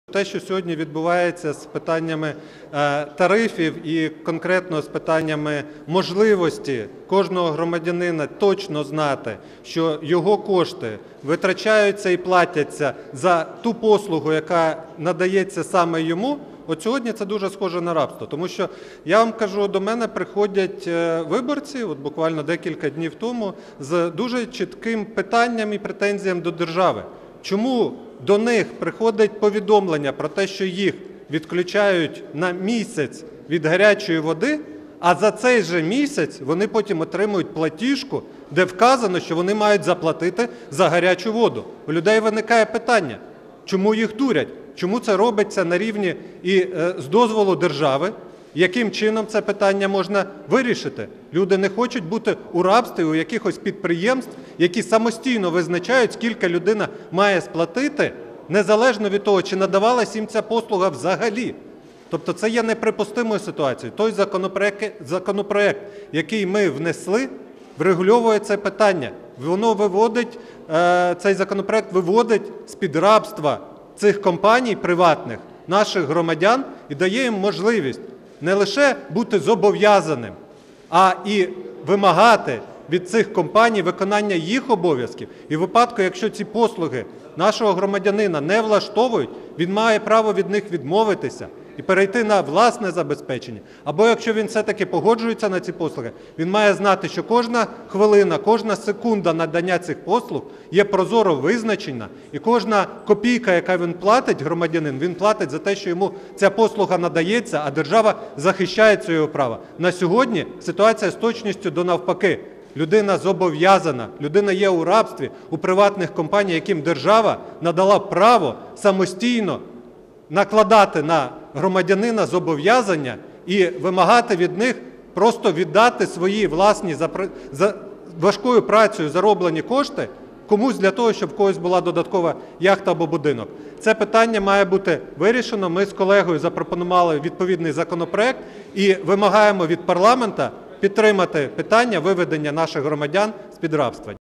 Українців треба звільнити від рабства компаній, які надають їм комунальні послуги, – Леонід Ємець
Українців необхідно звільнити від рабства приватних компаній, які надають громадянам комунальні послуги.

Про це заявив народний депутат фракції "Народний фронт" Леонід Ємець на брифінгу у Верховній Раді.

"Те, що сьогодні відбувається з питань тарифів та можливості кожного громадянина точно знати, що його кошти витрачаються і сплачуються саме за ту послугу, яка надається саме йому, дуже нагадує рабство", – зазначив народний депутат.

Леонід Ємець повідомив, що багато громадян звертаються до нього з дуже чітким питанням та претензією до держави – чому до них приходить повідомлення про припинення постачання гарячої води терміном на місяць, а потім вони отримують платіжку, де вказано, що вони мають заплатити за гарячу воду.

"Сьогодні людина перебуває в рабстві у приватних компаній, яким держава надала право самостійно накладати на громадянина зобов’язання та вимагати від нього віддати свої кошти, щоб у когось була нова яхта чи будинок", – сказав народний депутат.

"У людей виникає питання: чому їх дурять, чому це робиться з дозволу держави, і яким чином це питання можна вирішити. Люди не хочуть бути в рабстві в якихось підприємств, котрі самостійно визначають, скільки людина має сплатити незалежно від того, чи надавалася їй послуга взагалі", – пояснив представник "Народного фронту".

"Ця ситуація є неприпустимою", – наголосив Леонід Ємець.

За його словами, група народних депутатів внесла законопроект, який врегульовуватиме це питання - виводить громадян з-під рабства компаній, які надають послуги з постачання комунальних послуг та надає людям право бути не лише зобов’язаними, а й вимагати від цих компаній виконання своїх обов’язків. У випадку, якщо послуги не влаштовують громадянина, він може від них відмовитись.

"А якщо він все ж погоджується на такі послуги, - він може переконатися, що кожна хвилина надання йому цих послуг є прозоро визначеною, а самі послуги варті кожної сплаченої за них копійки", – додав народний депутат.

"Вимагаємо від парламенту підтримати питання виведення наших громадян з-під рабства", - закликав колег представник "Народного фронту".

Приєднуйся!